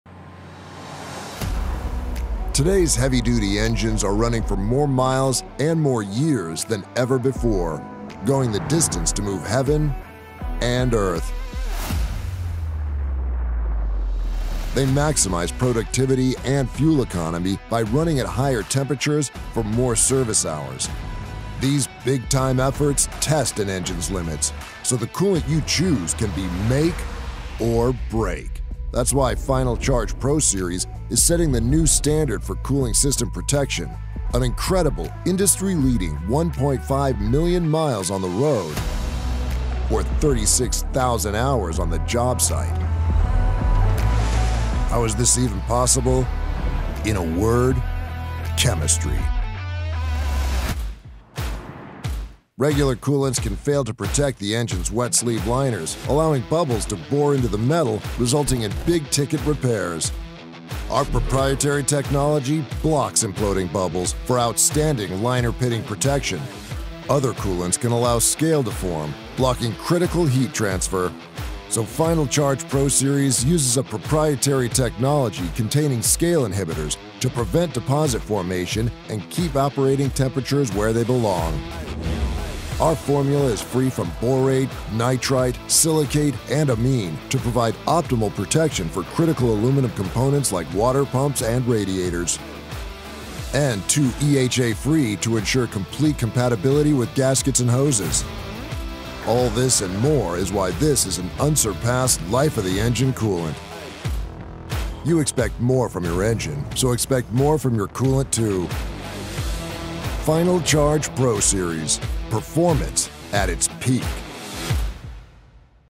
FINAL CHARGE PRO-SERIES Overview | PEAK Auto
Heavy Duty engines are running for more miles and years than ever. The commercial coolant you use can be make or break. See why FINAL CHARGE PRO-SERIES is the best coolant for the job.

This PEAK Auto video overviews the FINAL CHARGE PRO-SERIES as a solution for commercial antifreeze and coolant needs.